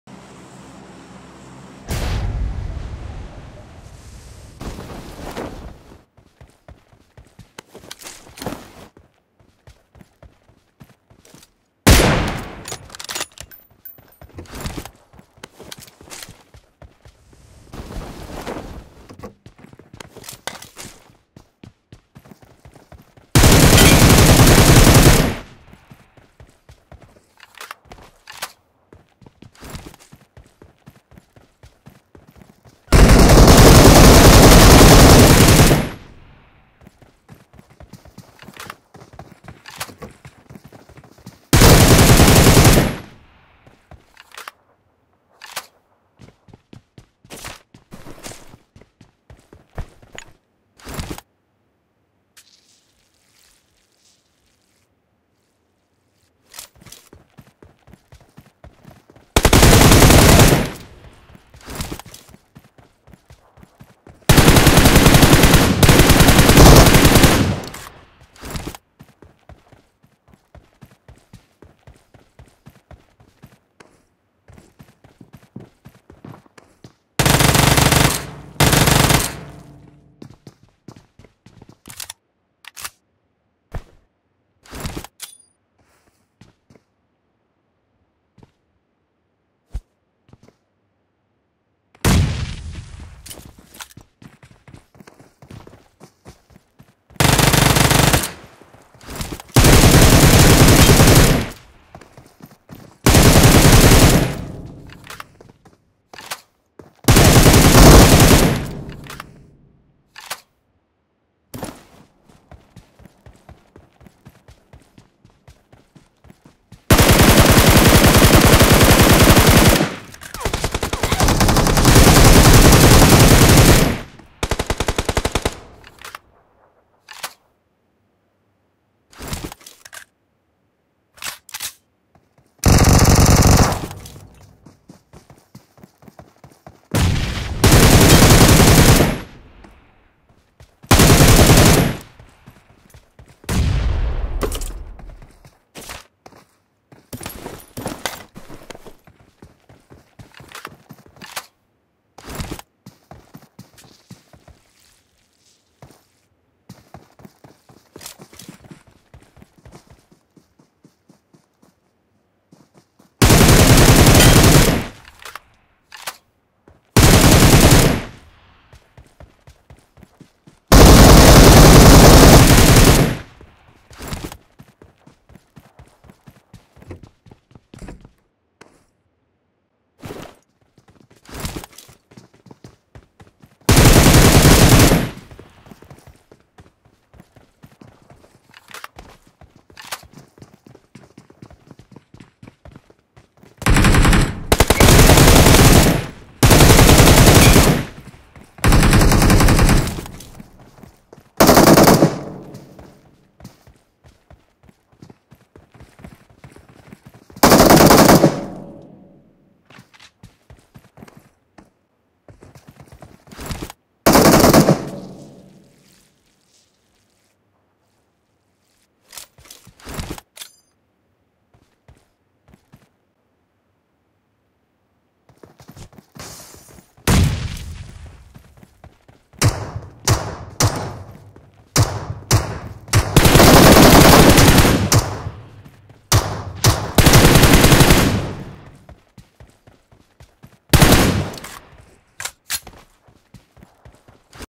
EXTREAM LEVEL INTENSE CLUTCHES 1VS5,1VS4,1VS3 BY FYME ASSASSIN AGAINST PRO SQUADS
Ignore Tags -